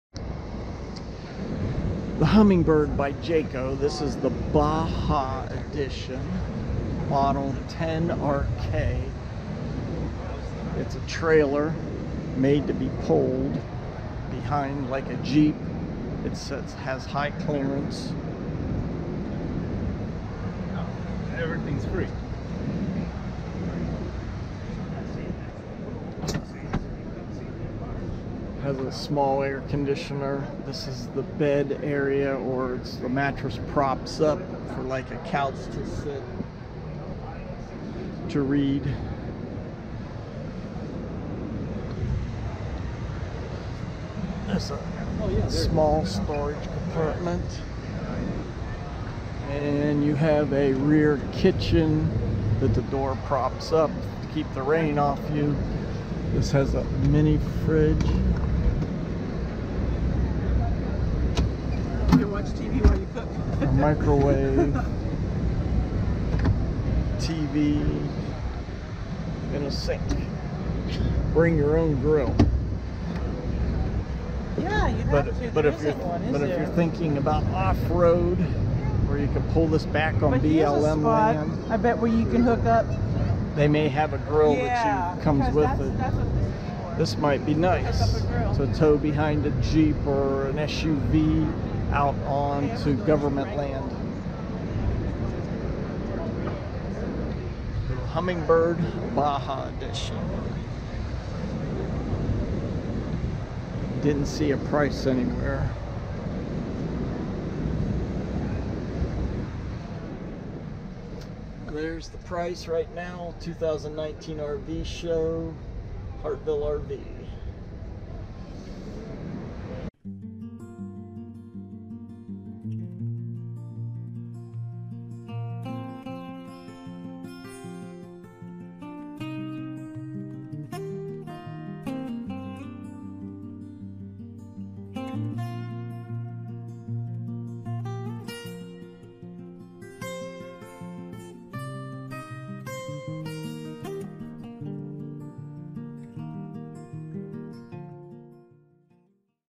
2019 Jayco Hummingbird
A visit through a Jayco Hummingbird trailer at the 2019 Ohio RV Supershow in Cleveland Ohio.